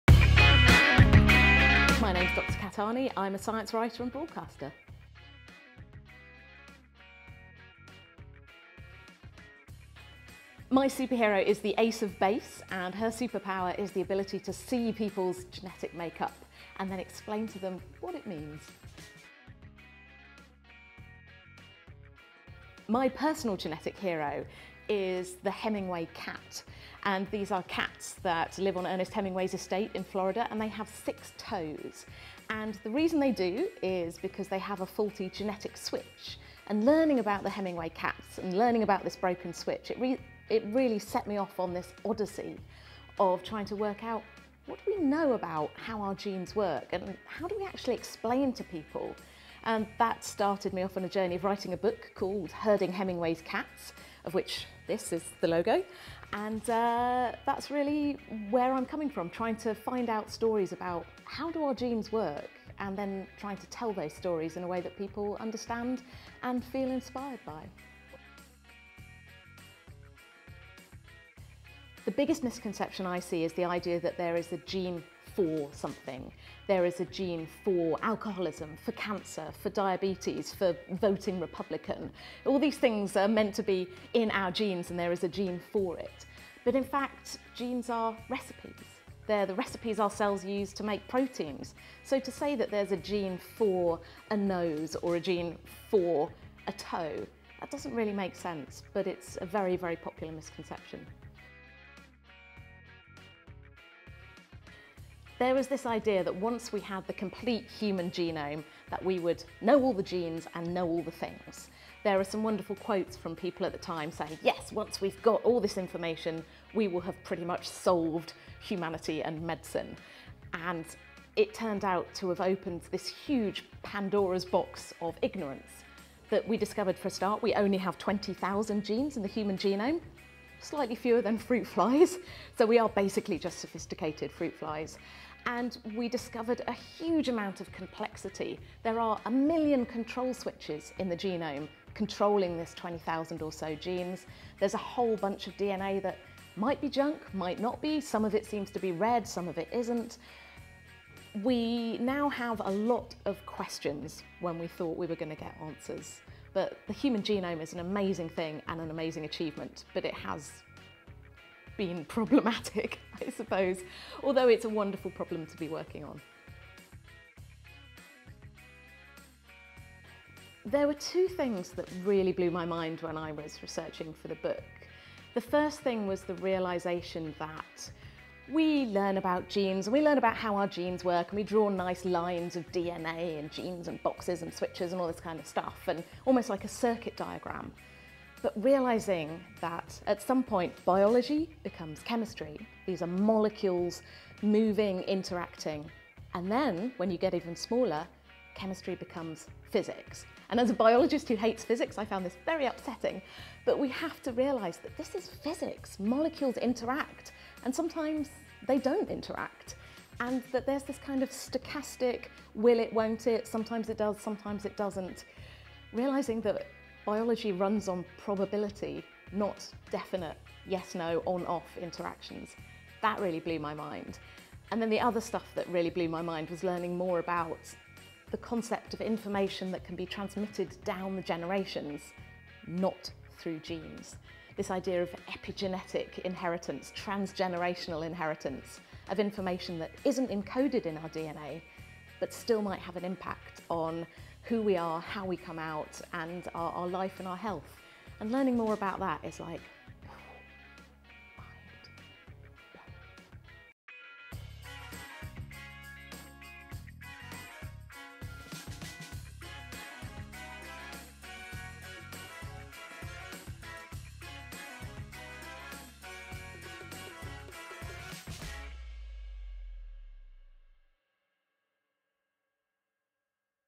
Dr Kat Arney - Cosmic Superheroes
Cosmic Superheroes is a unique photographic celebration of the superpowers it has taken for women at the forefront of the worlds of Arts and Science to establish themselves as superheroes in their individual fields. The exhibition will be free to attend at Conway Hall in London from December 8th to January 31st.

Dr Kat Arney is a science writer, speaker and
 broadcaster

A presenter on the Naked Genetics podcast, Kat is also a prolific public speaker and author of two books on genetics including the very well received Herding Hemingway’s Cats.

She is a regular writer on science topics for many leading UK papers and recently co-wrote and presented the BBC Radio 4 series Did the Victorians Ruin the World? Previously she spent many years as the media spokesperson for Cancer Research UK.

New huge live show at the Royal Albert Hall in June 2018: Robin Ince and Chris Hadfield's Space Shambles.

Cosmic Superheroes produced and curated by Trent Burton and Melinda Burton

Photography by Timothy Ginn

Hair and Make Up by Emily Mickelburgh

Support from Conway Hall and the March for Science London